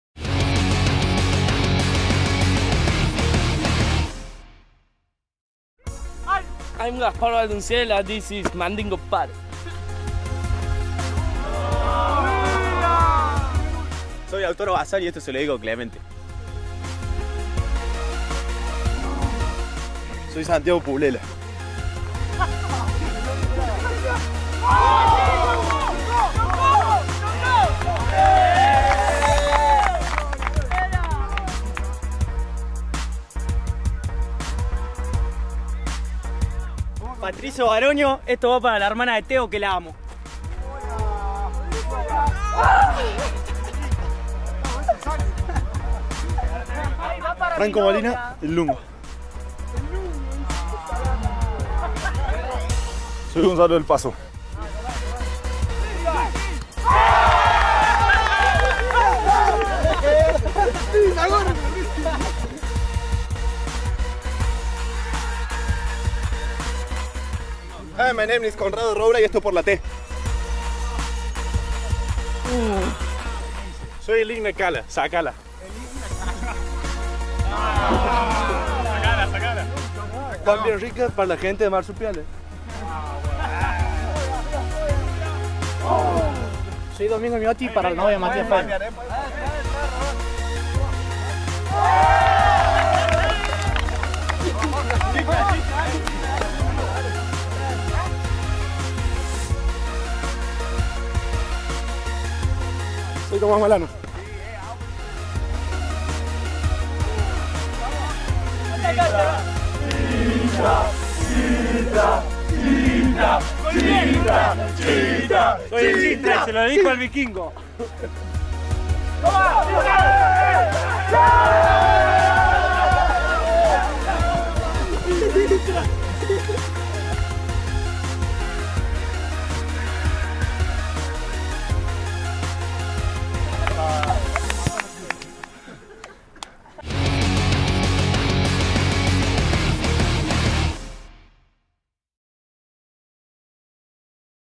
Los Pumitas se sumaron al Crossbar Challenge - Mundial Juvenil M20 de Rugby Inglaterra 2016
La selección Argentina juvenil de Rugby de menores de 20 años, se sumó al Crossbar Challenge. Fijate quiénes pudieron pasar la pelota por la H.